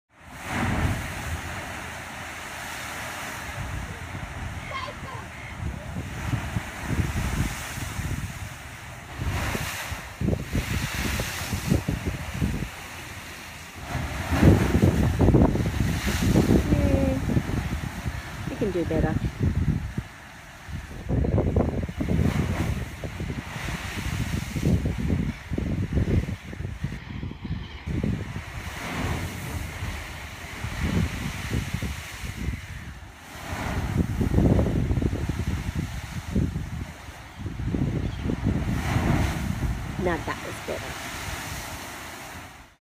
Kiama Blowhole
At least the blow hole was blowing today.  Some days there's not a breath of a fountain.